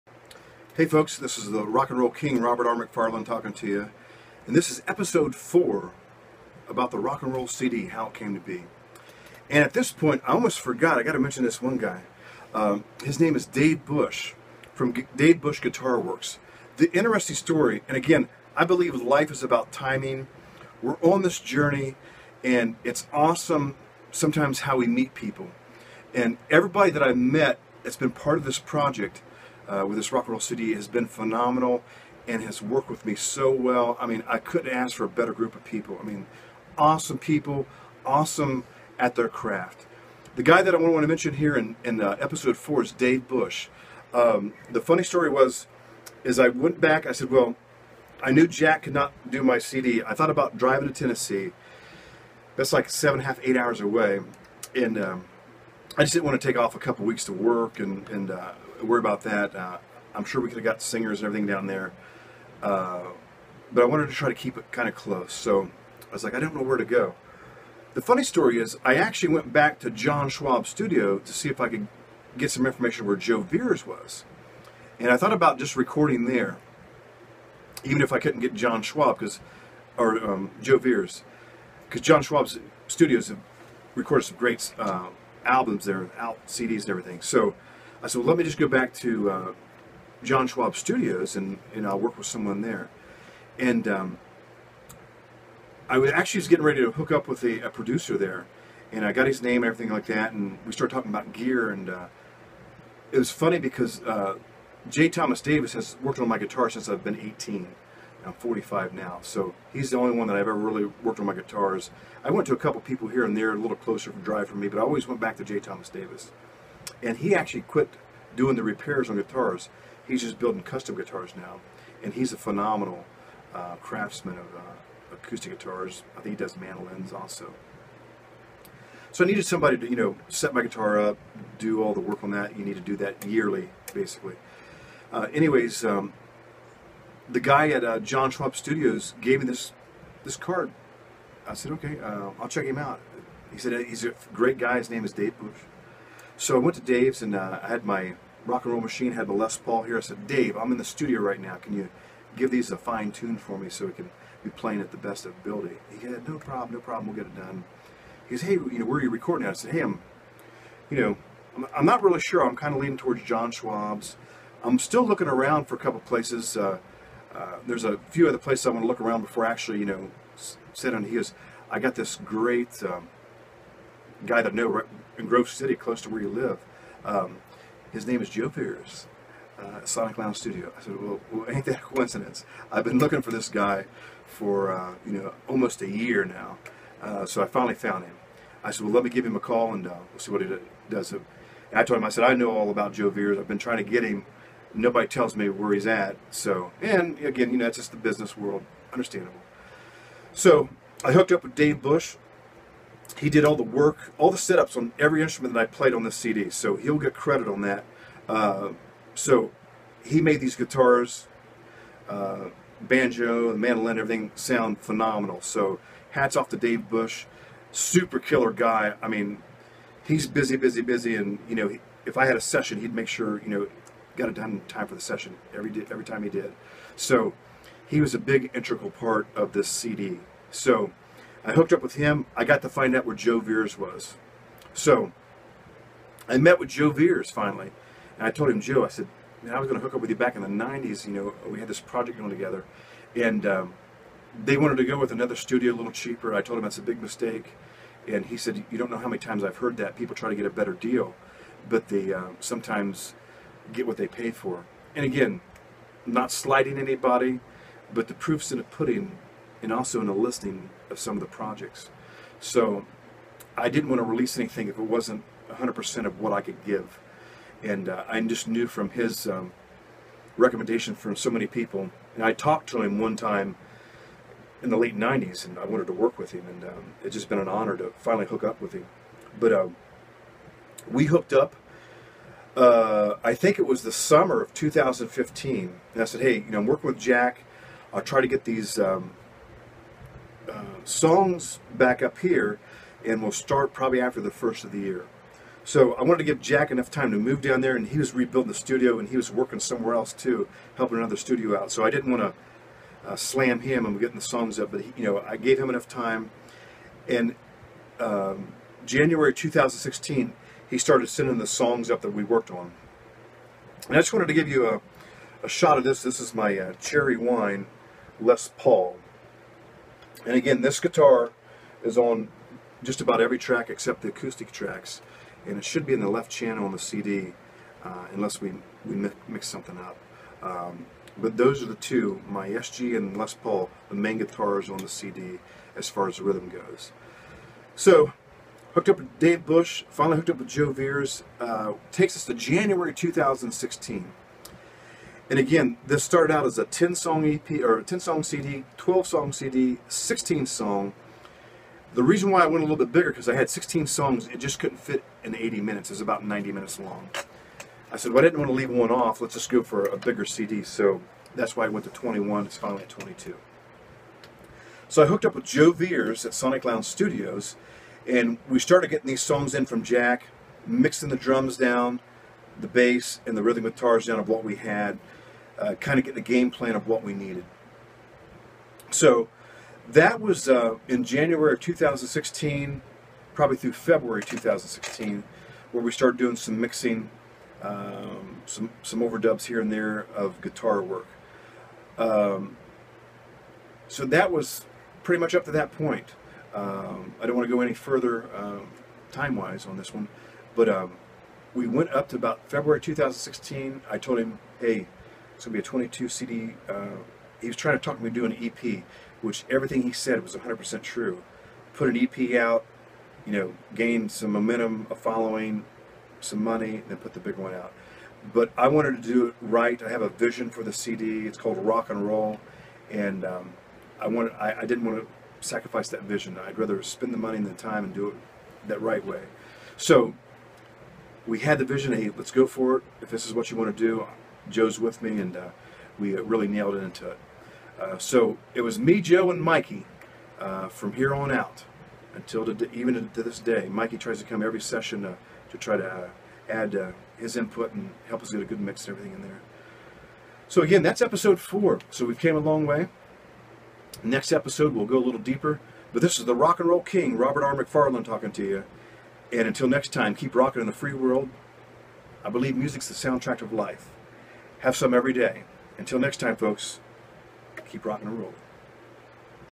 Episode 4 The Making of the Rock N Roll Cd. Dave Bush, the missing link to Joe Viers.
The Journey continues. We pick up with the story of how this CD started as a vision and became a reality and the people who helped me to fulfill a life long dream of doing an all original CD.